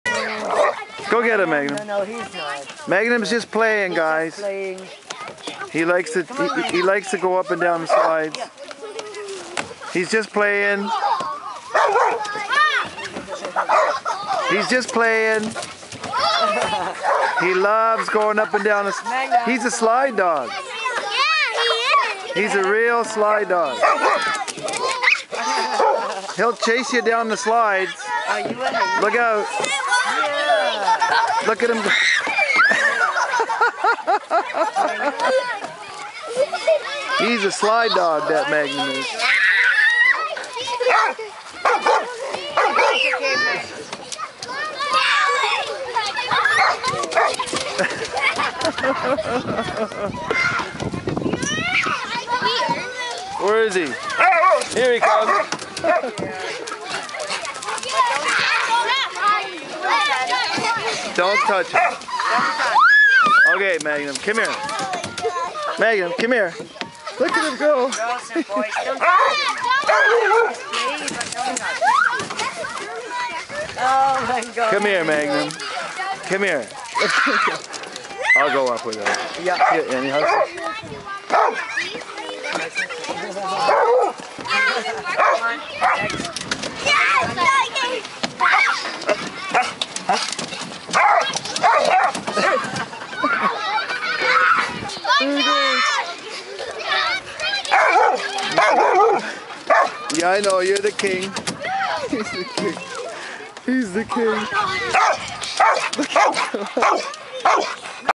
Prince Magnum Slide Dog.MP4
Pure bread Swiss Mountain Dog (Prince Magnum - now 4 years old) doing what he does best - herding children up and down slides in a playground in Ontario, Canada